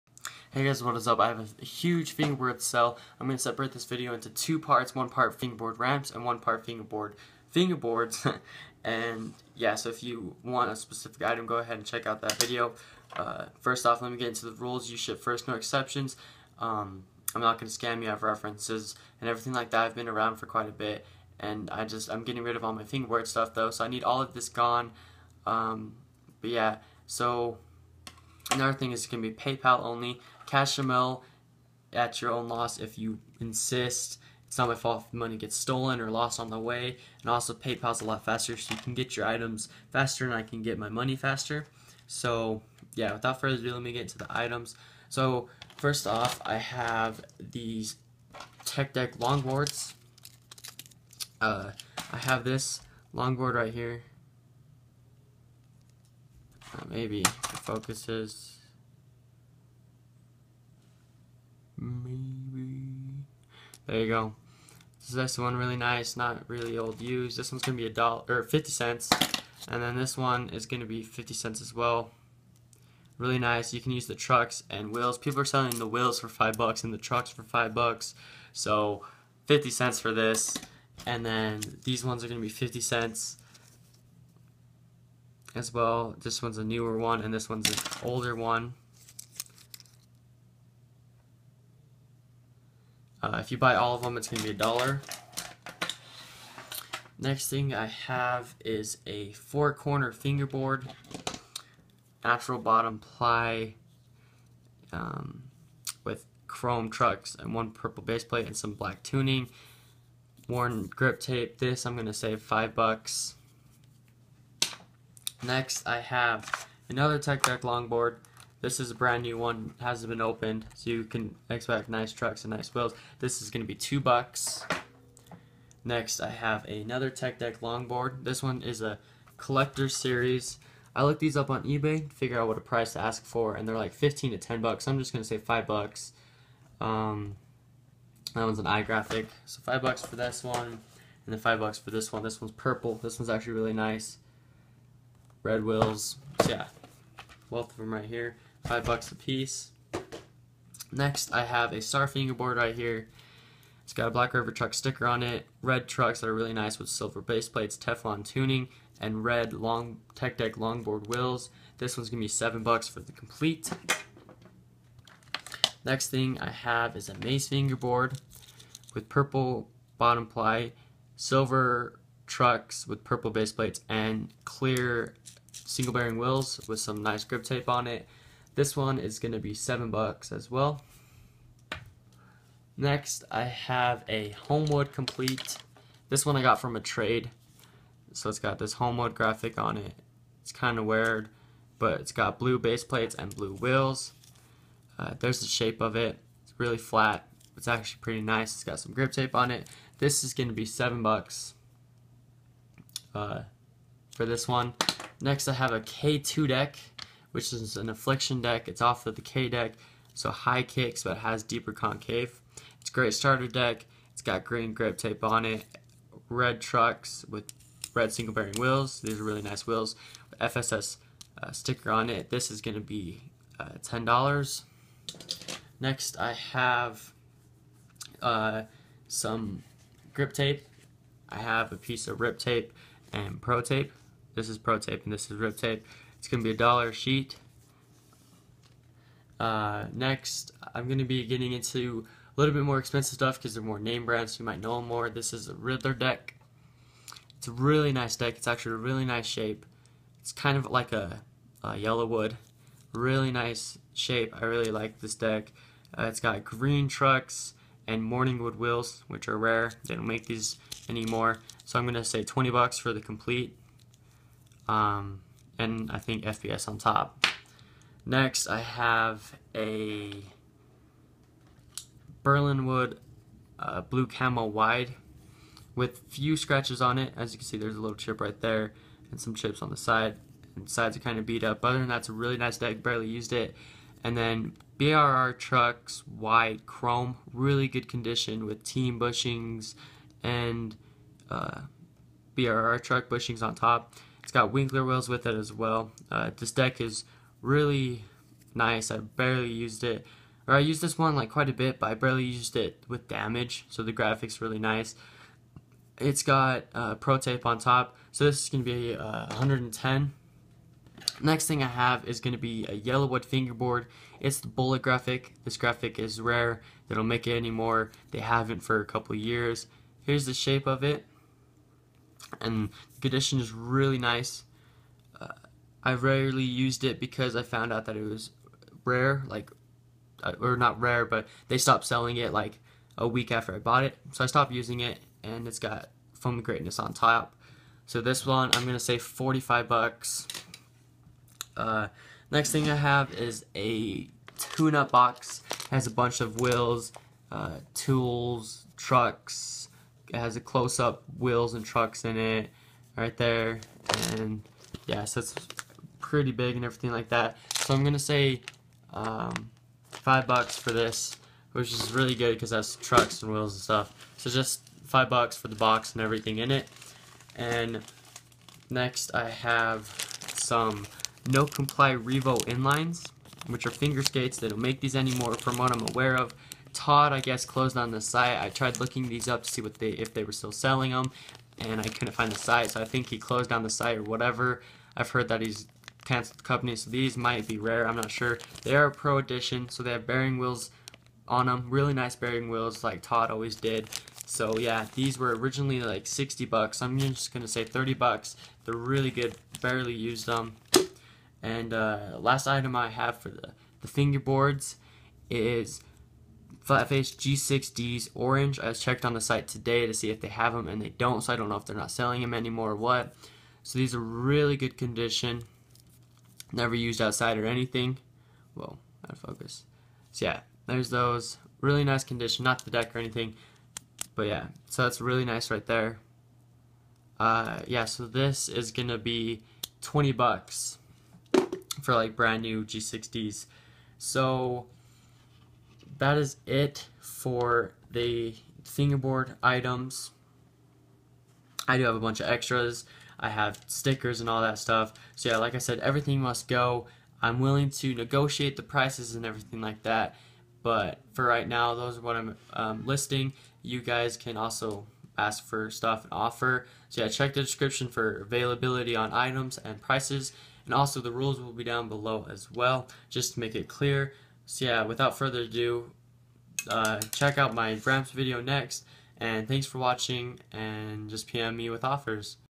HUGE fingerboard Sale (Yellowood Rare,Brr,etc.)
RULES: You send first no exception. I have refs.
PayPal only, IF cash in mail it's at your own risk.
I will take offers but NO trades
ITEMS:
Longboards- .50, .25, or 1.00 (HERE)
4CFB- 5.00 (HERE)
New Longboard- 2.00 (HERE)
New Collector Longboard 2- 5.00 (HERE)
Star Fingerboard- 7.00 (HERE)
Maze FB- 7.00 (HERE)
Homewood- 7.00 (HERE)
K2- 10.00 (HERE)
Tape 2- 1.00 (HERE)
Riddler Deck W/ Morningwood Wheels- 20.00 (HERE)
Blackriver Blue Camo Wide- 110.00 (HERE
REVO- 30.00 (HERE)
Yellowood Rare- 45.00 (HERE)